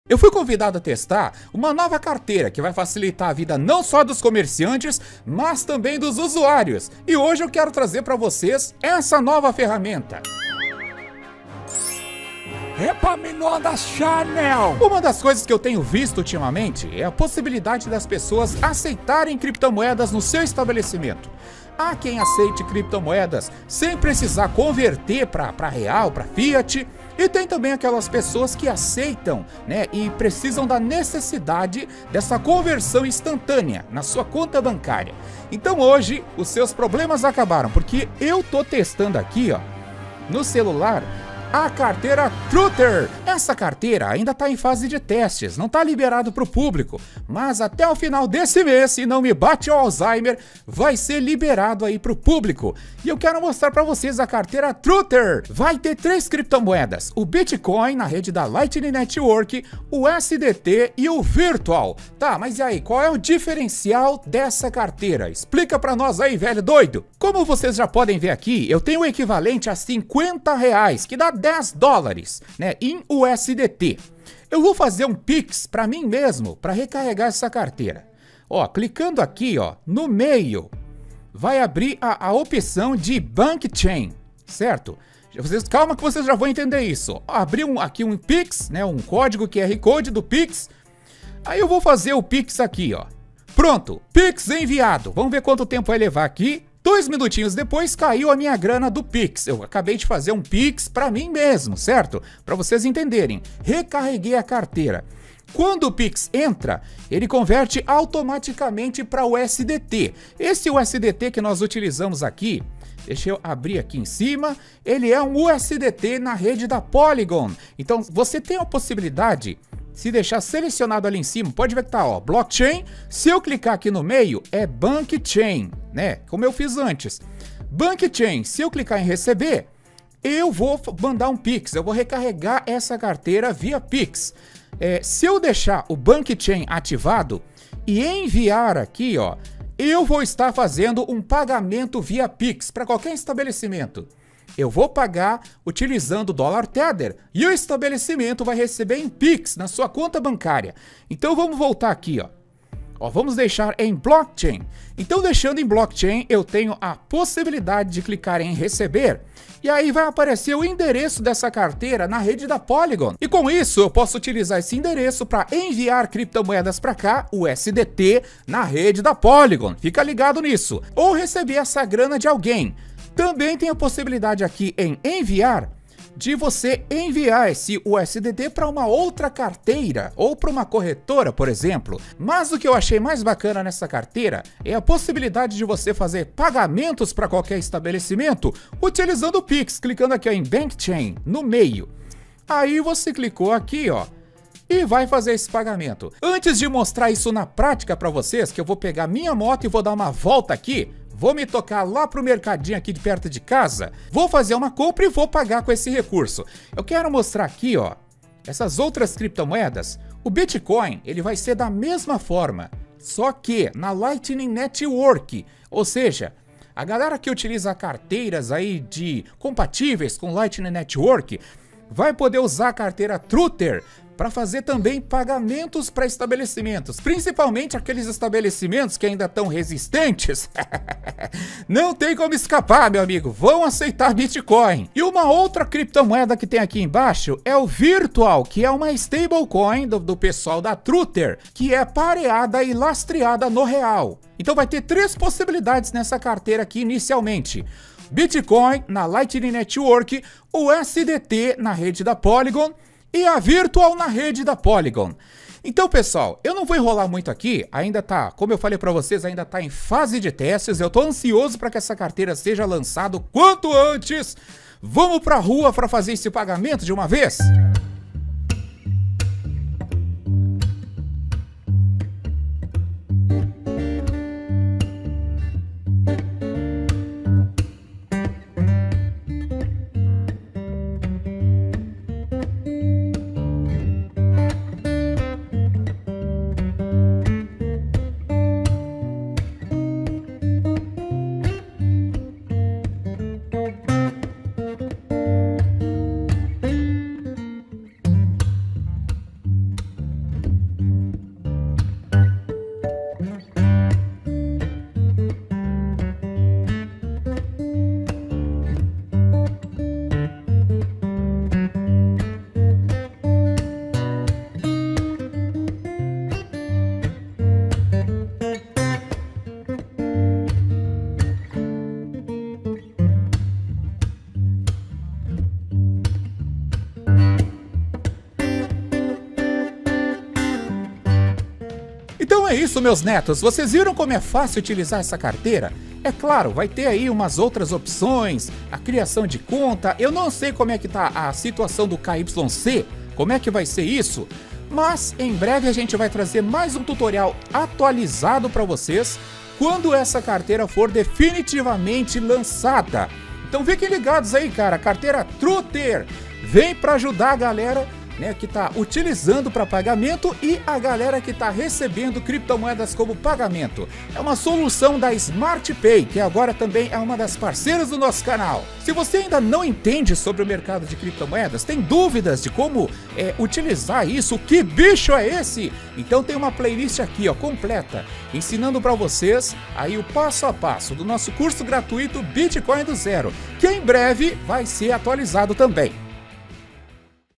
CARTEIRA TRUTHER BITCOIN NA LIGHTINING NETWORK DÓLAR TETHER
Você já pode utilizar a carteira truther para adquirir criptomoedas via pix ou pagar com criptomoedas nos estabelecimentos que tenham pix.